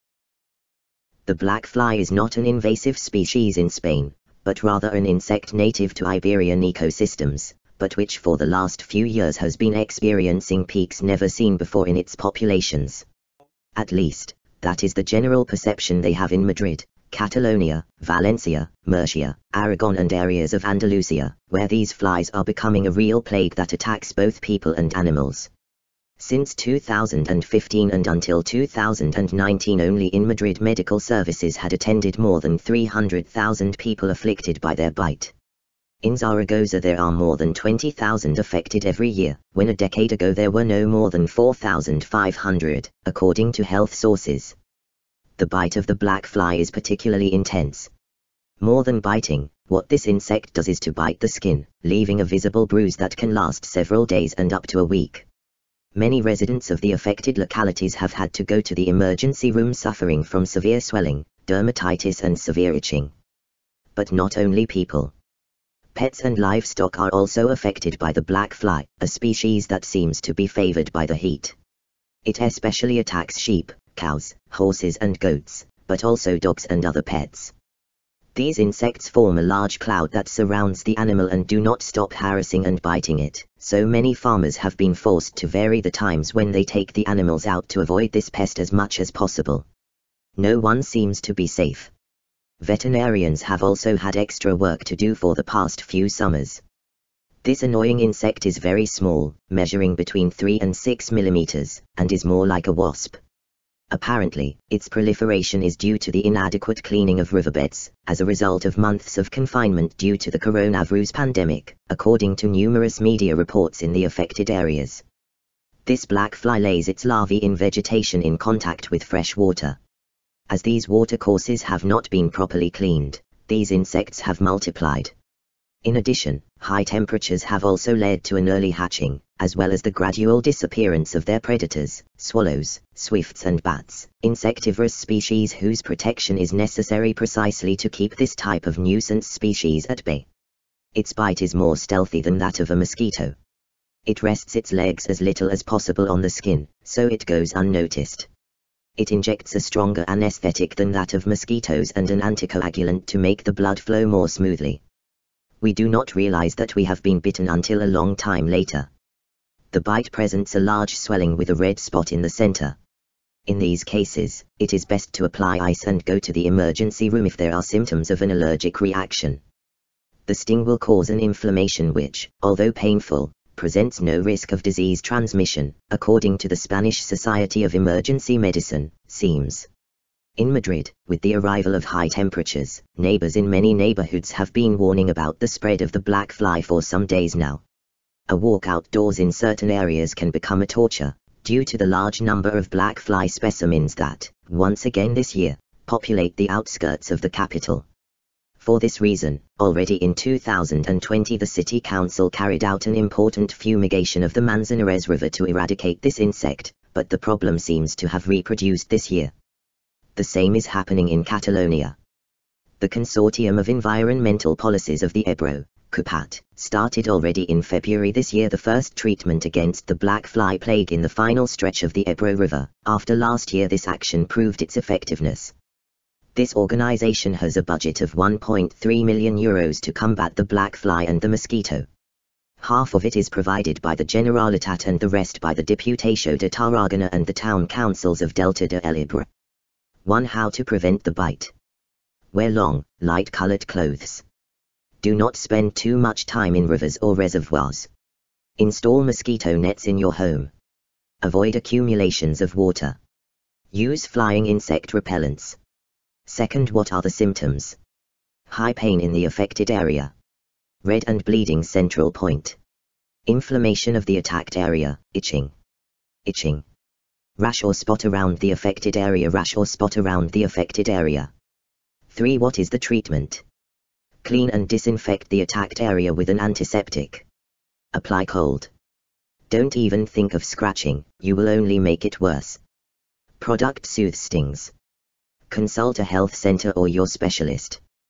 Insects | Black fly | Larvae that take over human skin | Invasive species | Stinging | Treatment
The black fly (Simulium erytrocephalum) is not an invasive species in Spain, but rather an insect native to Iberian ecosystems, but which for the last few years has been experiencing unprecedented peaks in its populations. At least, that is the general perception in Madrid, Catalonia, Valencia, Murcia, Aragon and parts of Andalusia, where these flies are becoming a real plague that attacks both people and animals.